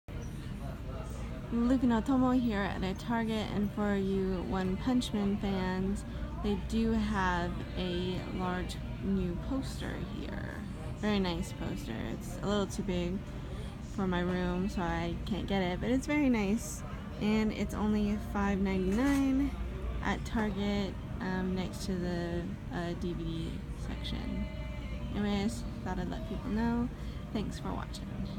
One Punch Man poster at Target
they have a fair amount of posters at Target - i found the One Punch Man poster towards the back of the wall in the DVD section

i also made a video about the Pirates of the Caribbean 5 movie poster - which was closer to the front of the DVD section but i didn't notice they had more posters in the back

so, best to look around for posters - they have em hidden everywhere X3